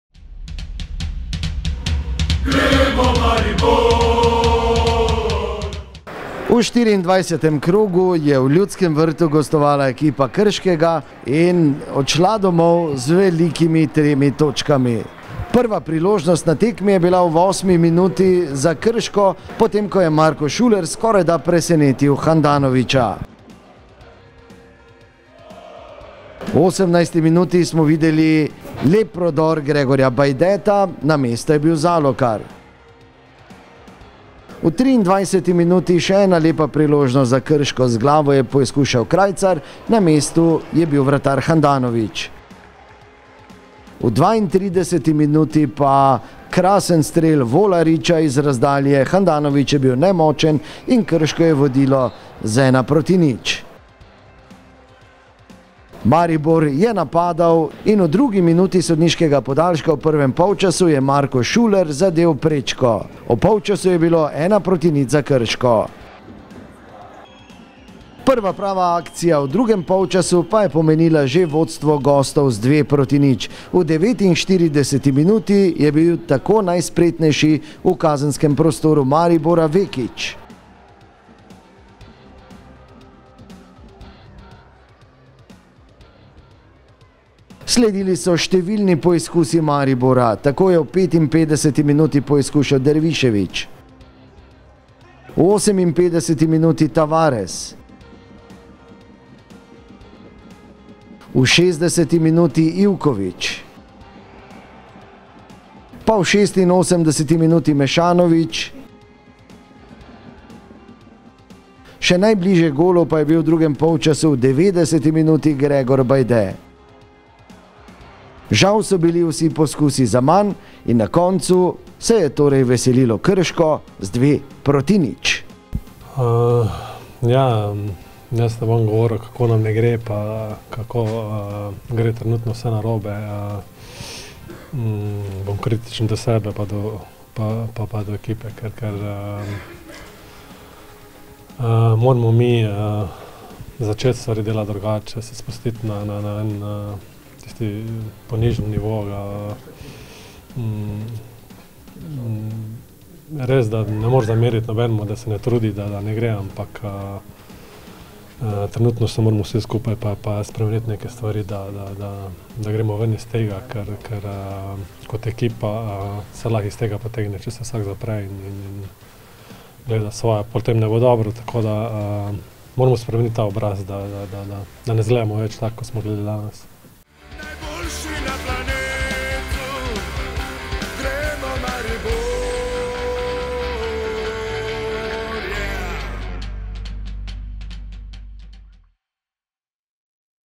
MARIBOR - KRŠKO 0:2 (0:1)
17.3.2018 Maribor, stadion Ljudski vrt. Prva Liga Telekom Slovenije, 24. krog. Glavni sodnik: Rade Obrenović (Ljubljana).